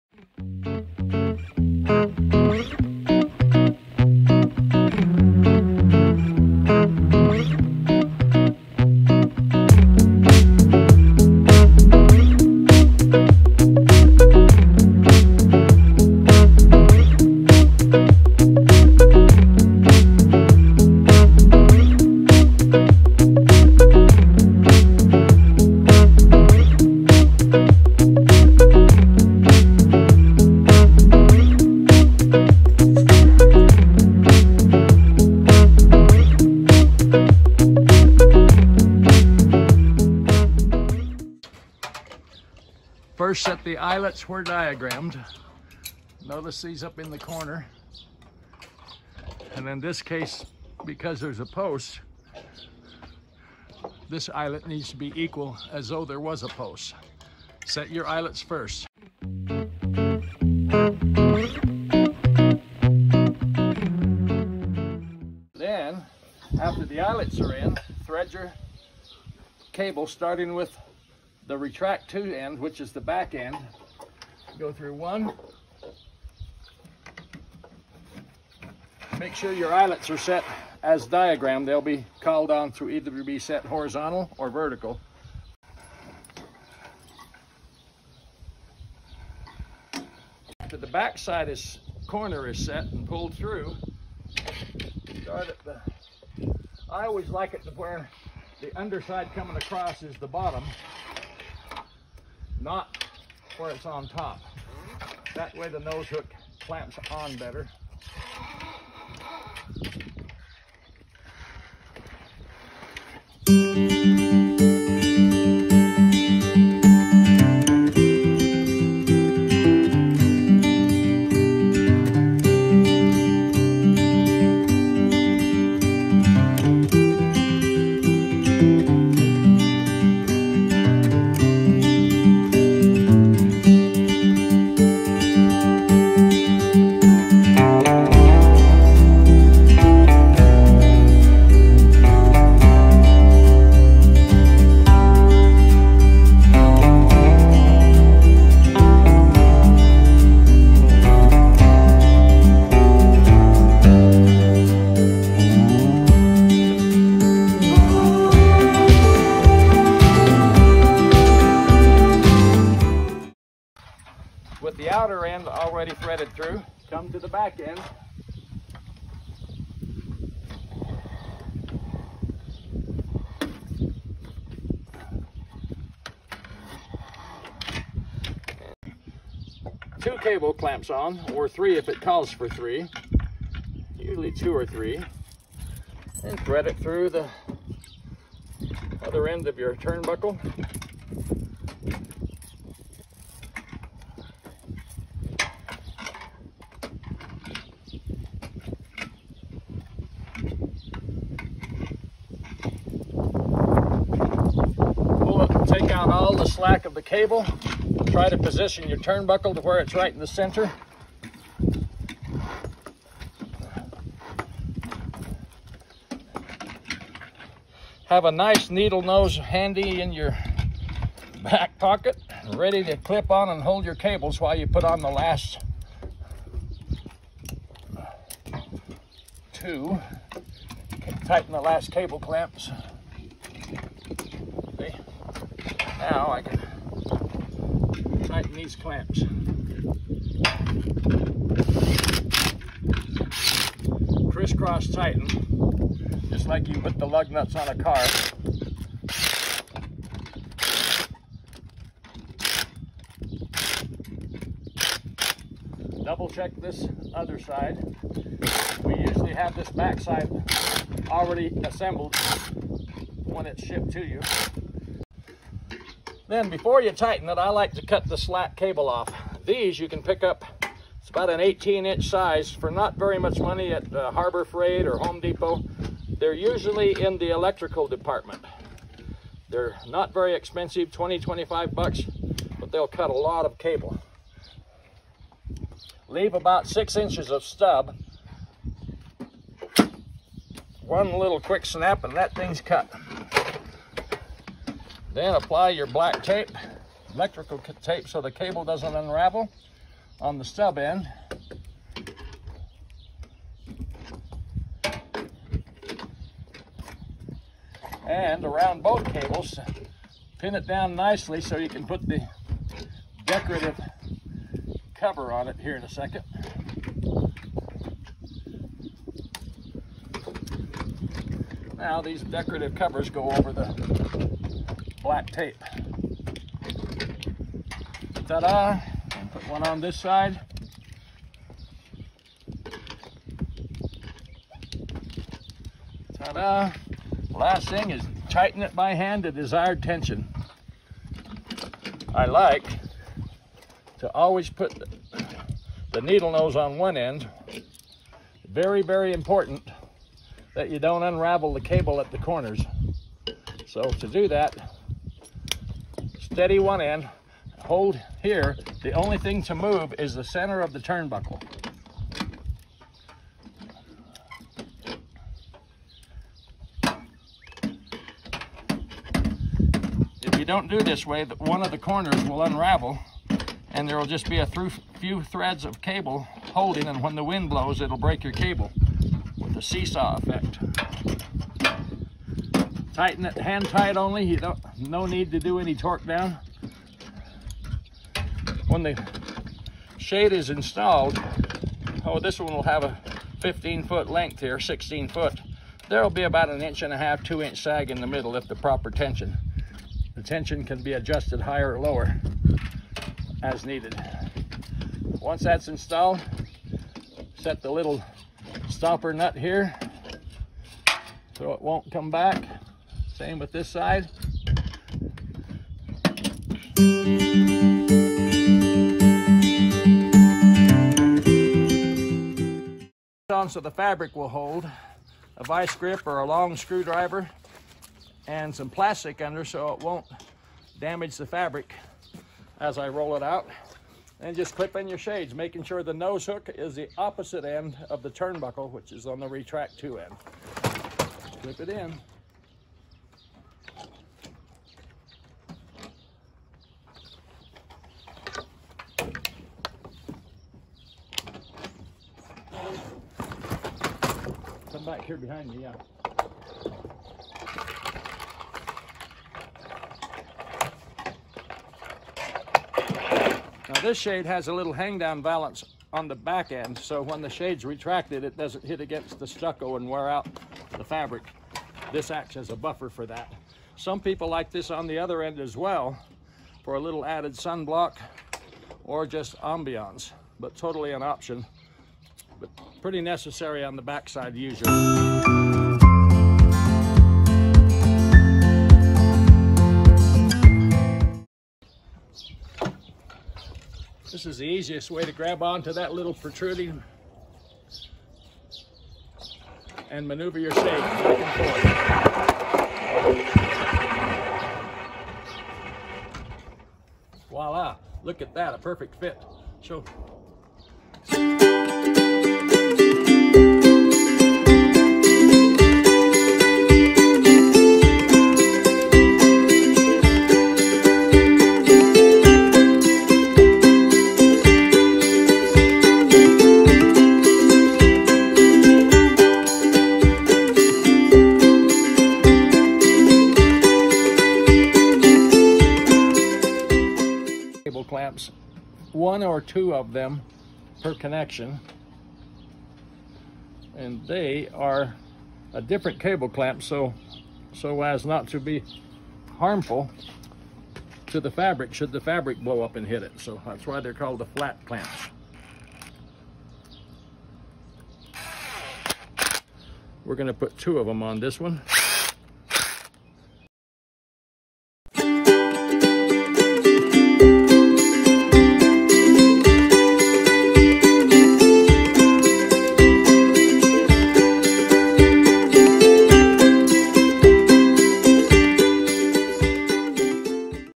How to easily install your Retractable CableShade system!
Choose from hundreds of great top-grade fabrics to create your perfect home or business shade solution!  Installation is extremely easy, and can take as little as 30-40 minutes per shade.  for more information.